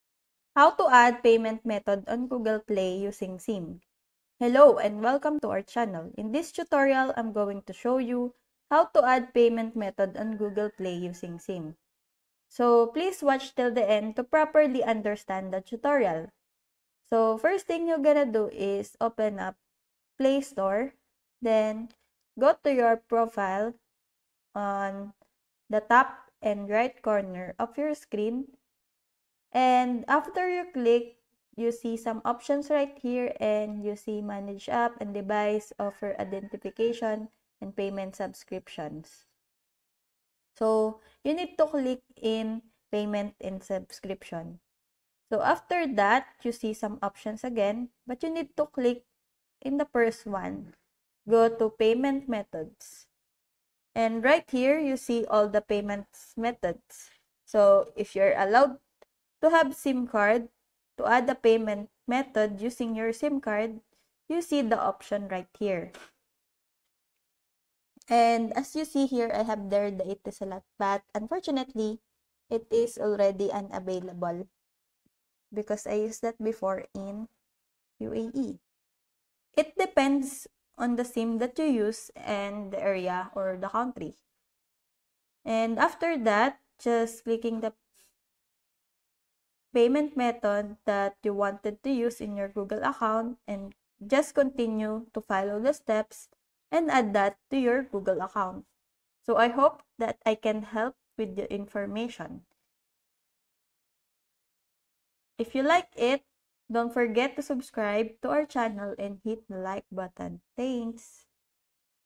How To Add Payment Method On Google Play Using Sim (NEW*)
In this video I'll show you How To Add Payment Method On Google Play Using Sim. The method is very simple and clearly described in the video. Follow all of the steps & learn How To Add Payment Method On Google Play Using Sim!